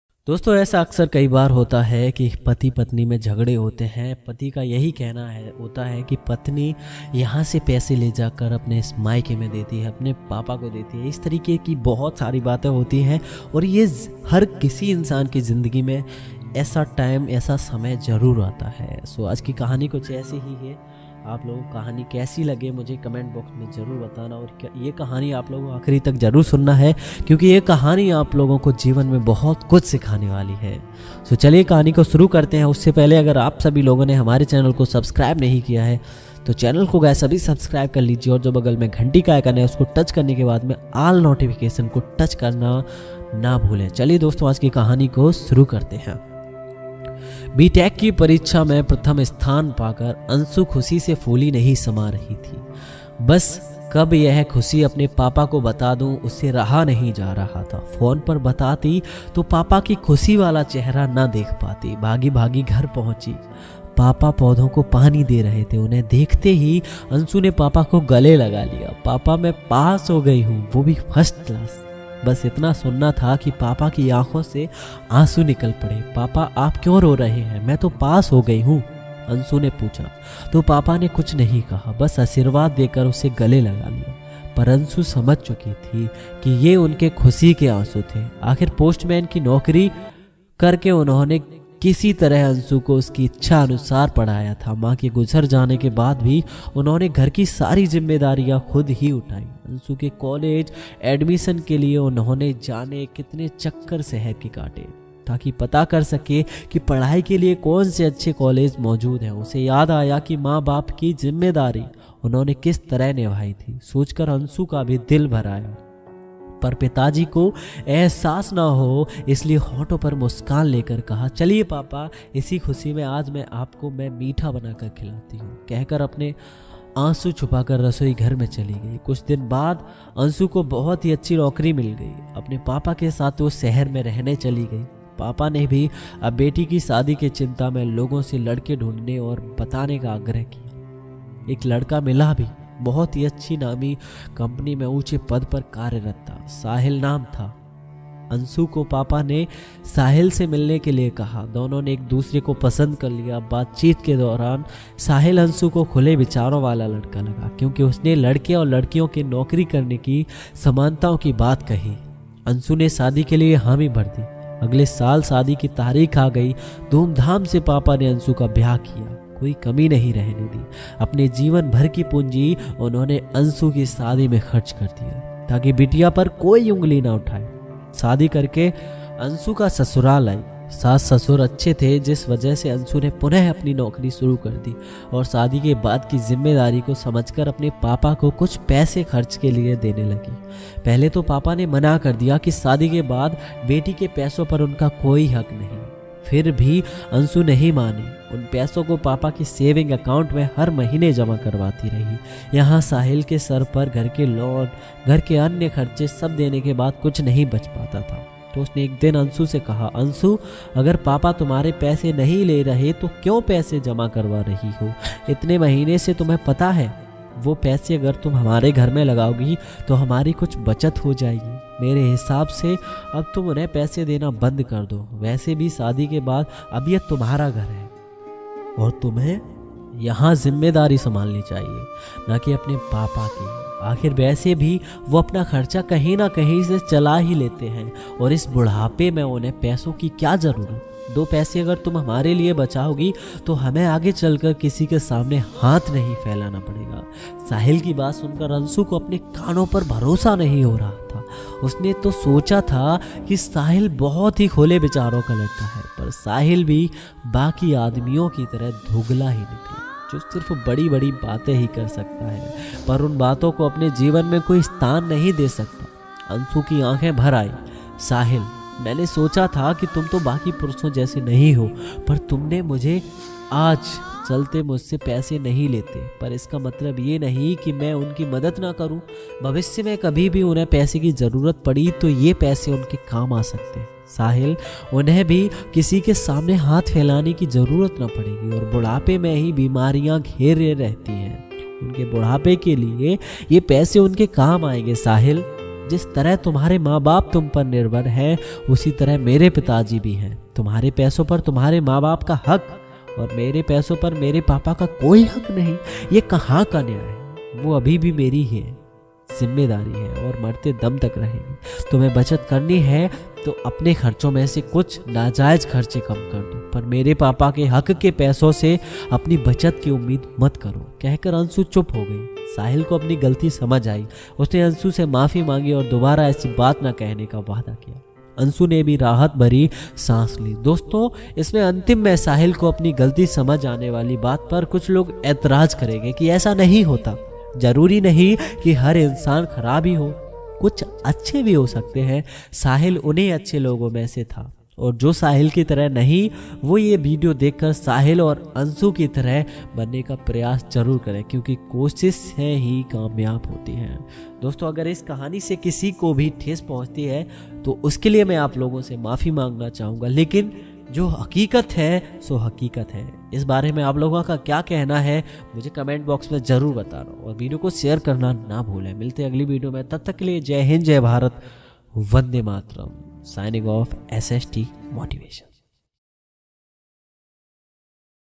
Best Life Changing Story For Husband Wife By Shivam Tomar / Husband Wife Real Life Story
MY SETUP-
My Microphone
My Laptop
My Second Microphone
My Phantom Power